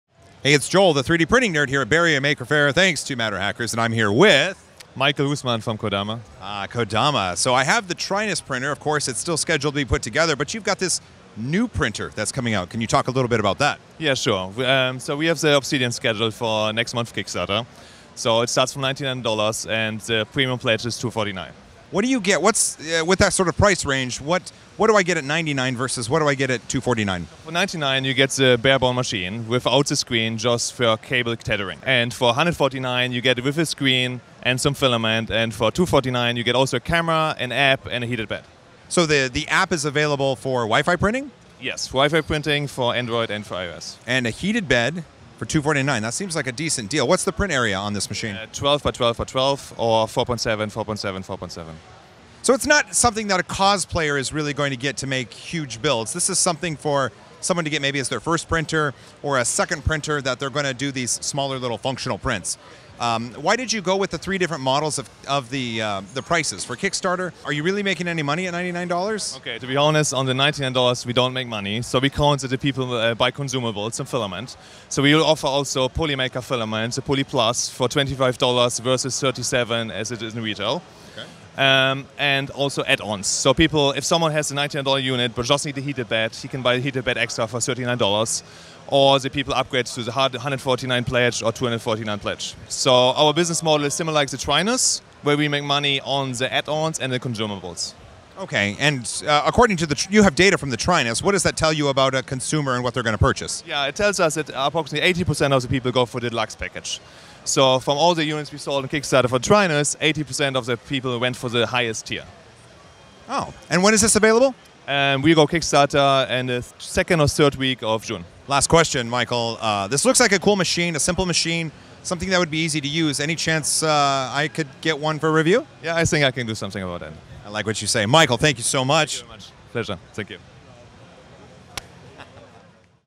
The Kodama Obsidian / $99 3D Printer at Bay Area Maker Faire
I had the chance to speak with Michael from Kodama while at Bay Area Maker Faire. They are launching the Obsidian on Kickstarter, a $99 3d printer with optional upgrades.

509 NE 165th st
Shoreline, WA 98155
USA